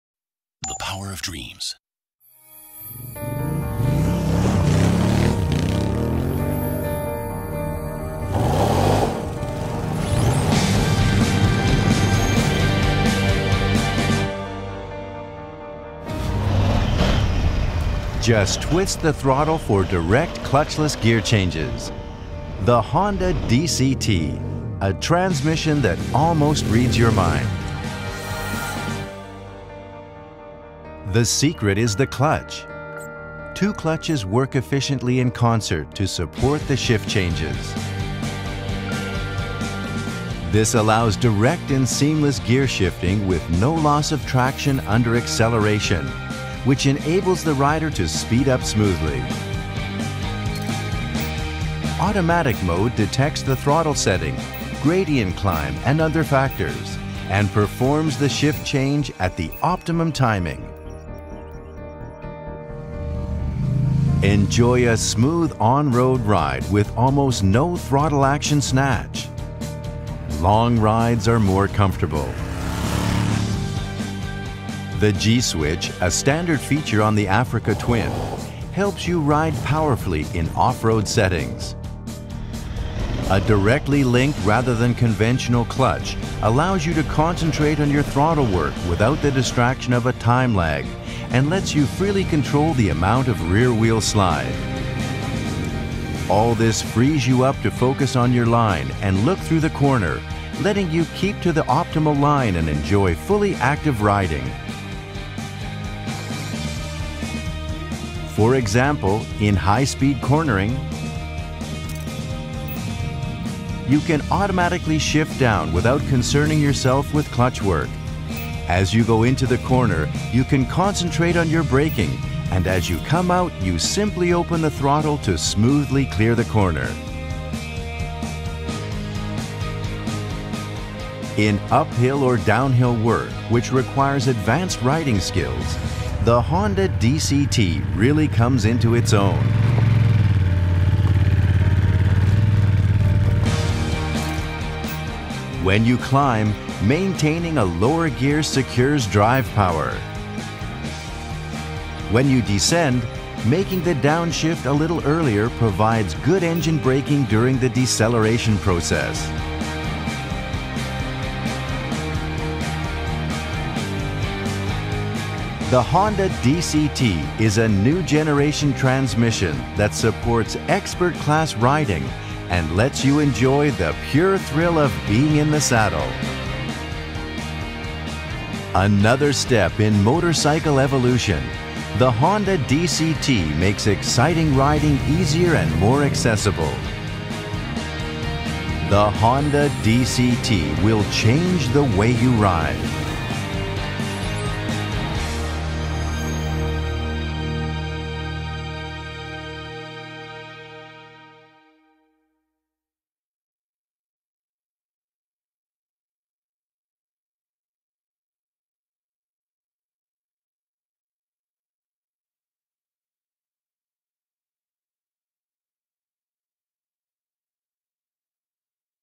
Neredeyse Zihninizi Okuyabilen Şanzıman Sistemi (DCT)
Dünyada ilk olan Honda Çift Kavramalı Şanzıman (DCT), motosikletin evriminde yeni bir adım! Honda'nın geliştirdiği yeni nesil çift kavramalı şanzıman (DCT), macera sizi nereye sürüklerse sürüklesin daha konforlu ve keyifli bir sürüş yaşatır.